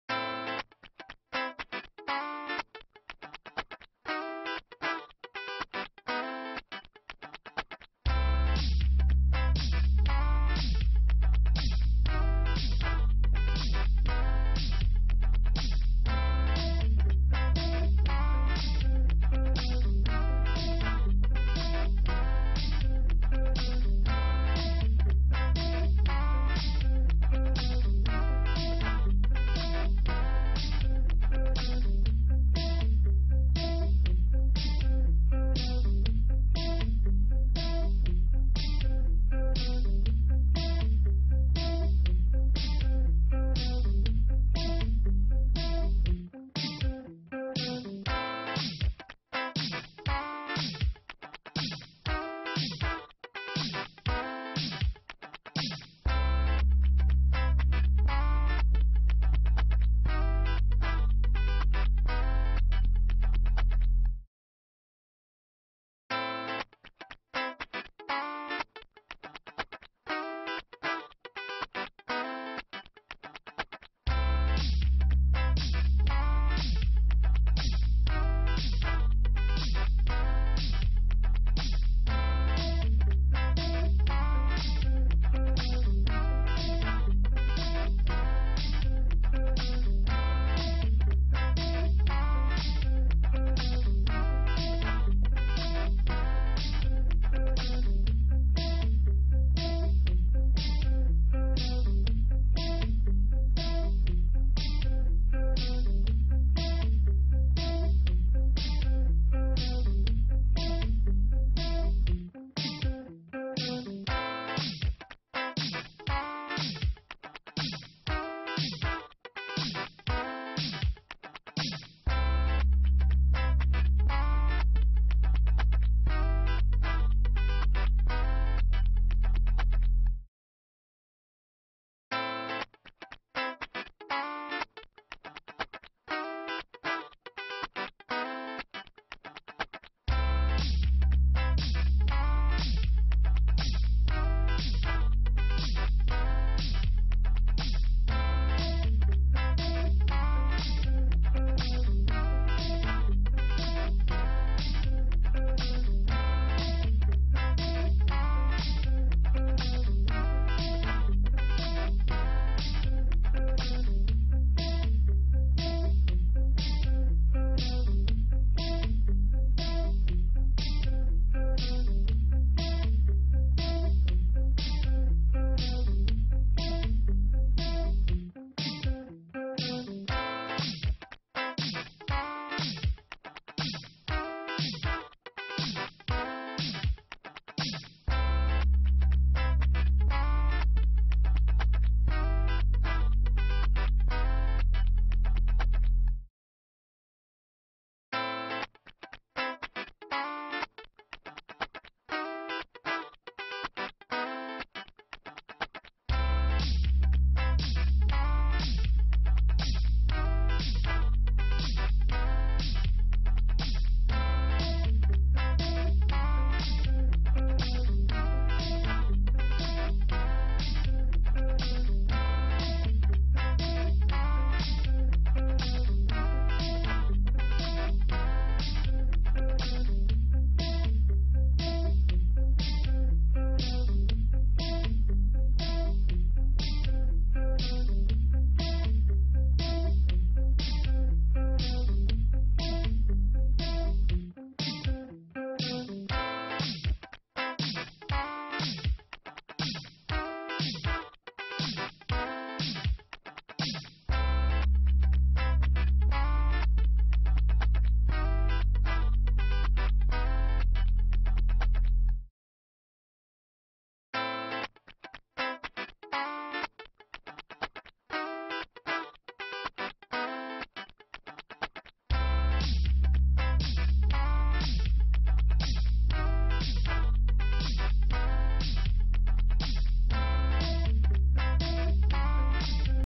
2016 Forest River SANDPIPER 370RDOK Fort Worth, Dallas, Plano, Frisco, McKinney, TX UFW1309
USED 2016 Forest River SANDPIPER 370RDOK available in Fort Worth, Texas at United RV. Servicing the Dallas, Plano, Frisco, McKinney, TX area.
2016 Forest River SANDPIPER 370RDOK  - Stock#: UFW1309 - VIN#: 4X4FSAP2XGJ033216

United RV
5100 Airport Freeway

Fiber glass exterior,,5 Slides,,Power awning,,50 Amp service,,Cab/sat prep,,Out side shower,,Out side speakers,,Out side kitchen,,TV Antenna,,10 Gal hot water heater,,16&amp;quot; Tires w/ alum wheels,,2 30 lb propane tanks,,Ladder,,,,